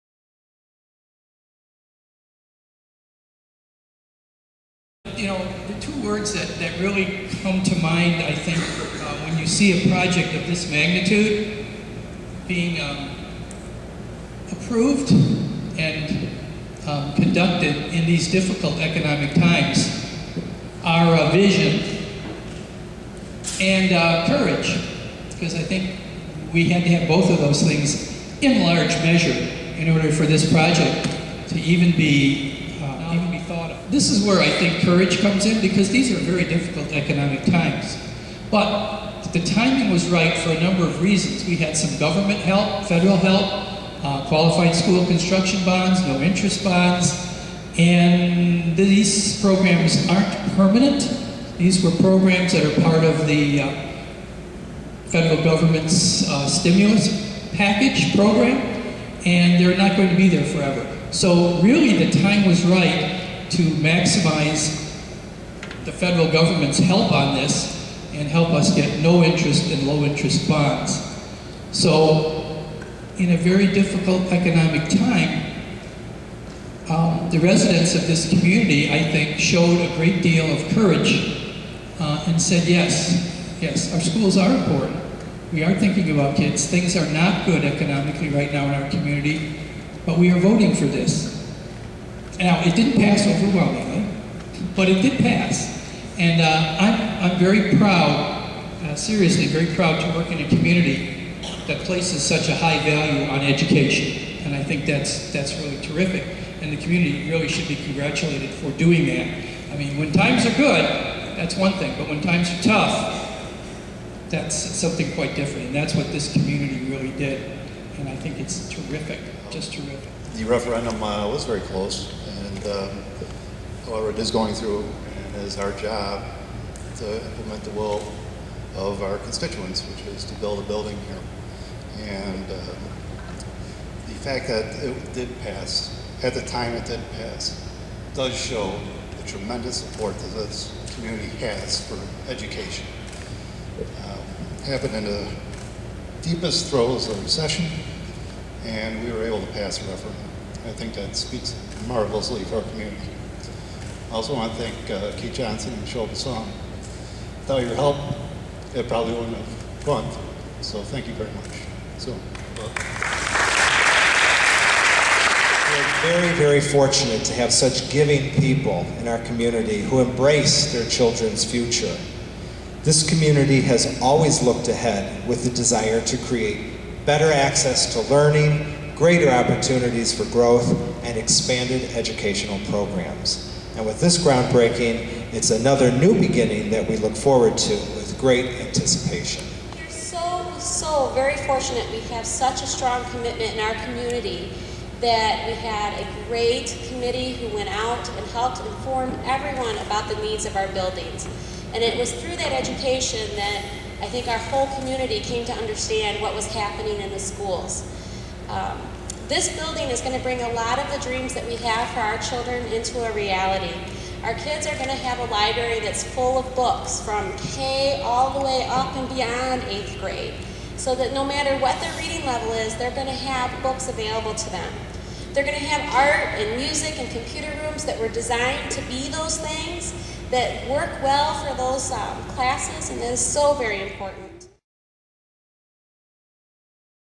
westofthei.com
Trevor-Wilmot (Wis.) School District officials comment at the Trevor School addition groundbreaking ceremony Sept. 7, 2010.